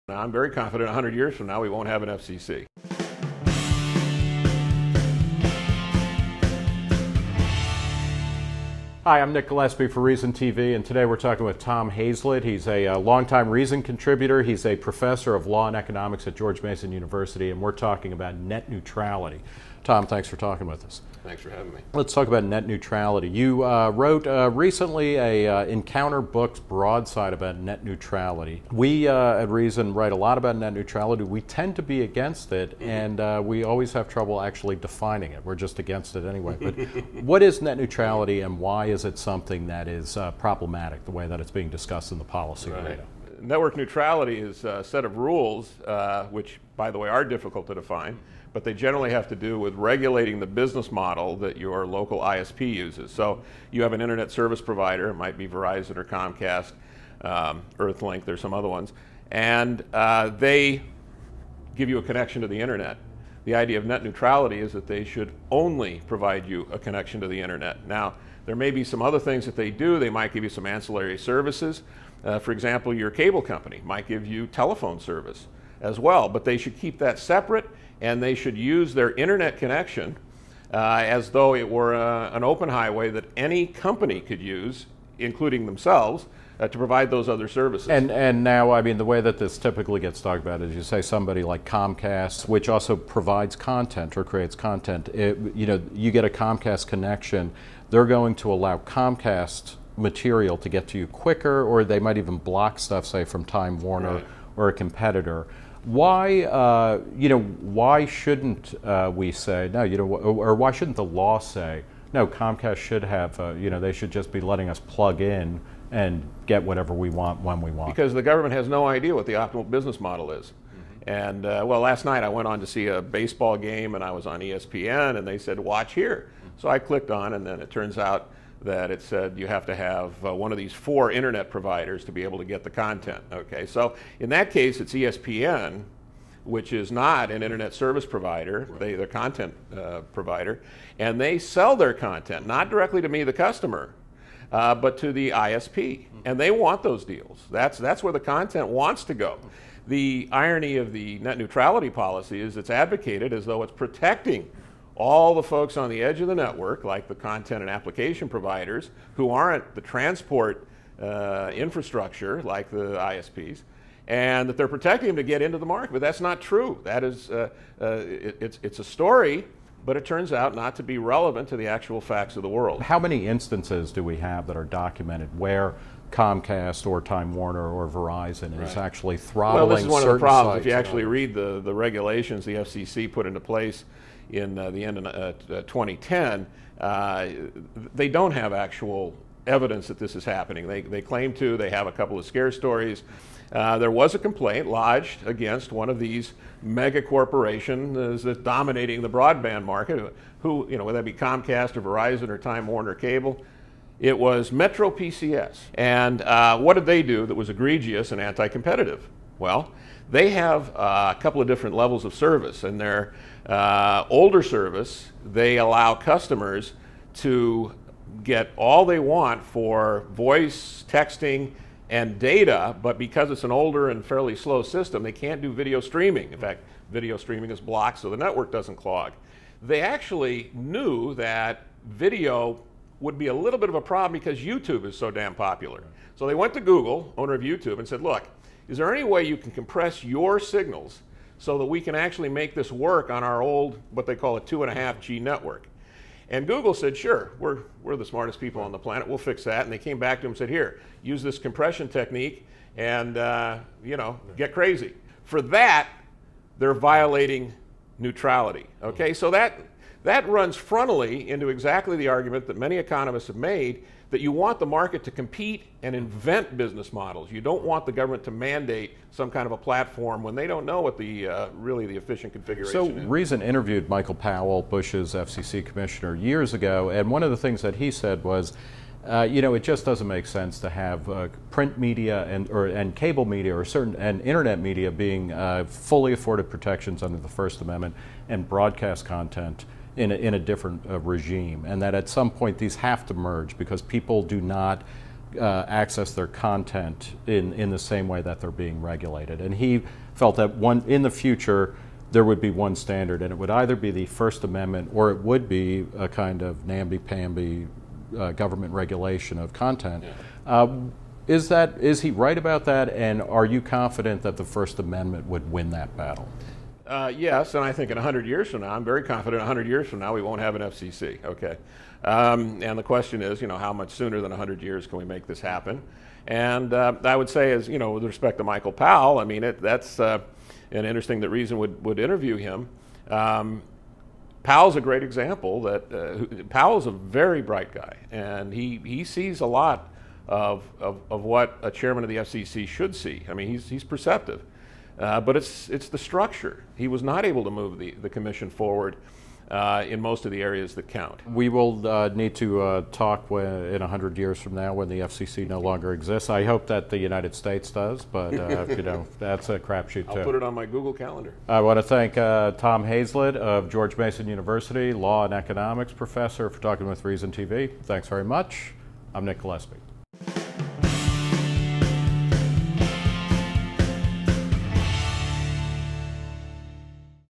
The Fallacy of Net Neutrality: Thomas Hazlett on the FCC & Consumer Protection
"I'm very confident a hundred years from now we won't have an FCC," says Thomas Hazlett, Reason contributor and George Mason economics professor.

Internet service providers are coming under scrutiny from both the FCC and net neutrality supporters who want to ensure unrestricted consumer access to the Web. However, Hazlett points out that the fear over ISPs limiting Web content is unfounded and government "has no idea what the optimal business model is" to effectively regulate.

Hazlett sat down with Reason TV's Nick Gillespie to discuss net neutrality, the Internet, and and his Encounters Broadside book "The Fallacy of Net Neutrality."

Camera by Meredith Bragg and Josh Swain. Edited by Swain.